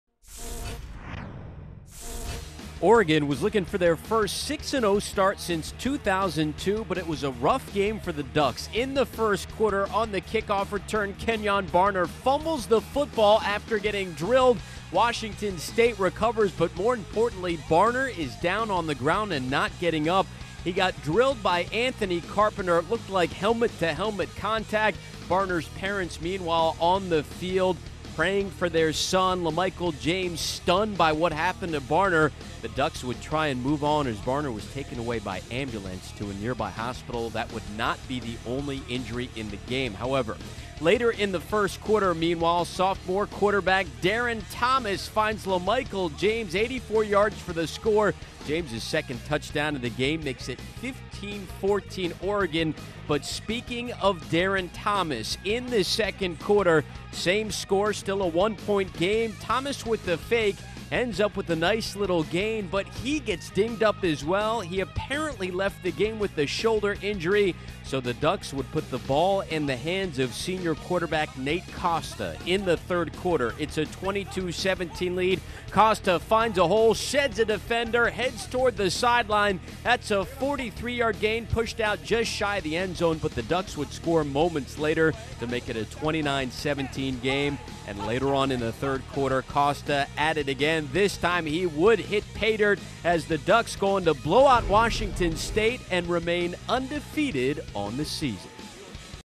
No. 3 Oregon 43 Washington State 23 PAC 10 -- 2010
LaMichael James scored 3 touchdowns, one on an 84-yard scoring reception, and No. 3 Oregon shook Washington State in the second half for a 43-23 victory Saturday. Coming off a big home win over Stanford that flew the Ducks past Boise State in the AP Top 25 poll, Oregon struggled to hold off the pesky Cougars.

Backup quarterback Nate Costa played the final 3 quarters for Oregon (6-0, 3-0 Pac-10), leading 3 scoring drives after Darron Thomas left because of an injury to his right shoulder. Thomas said afterward he could have come back but was held out for precautionary reasons. The Ducks also were without backup running back Kenjon Barner for most of the game.

He was flattened on a kickoff return by Anthony Carpenter, fell to the turf and trainers tended to him for more than 10 minutes. He walked with assistance to a stretcher and was taken to a hospital, where he was in stable condition.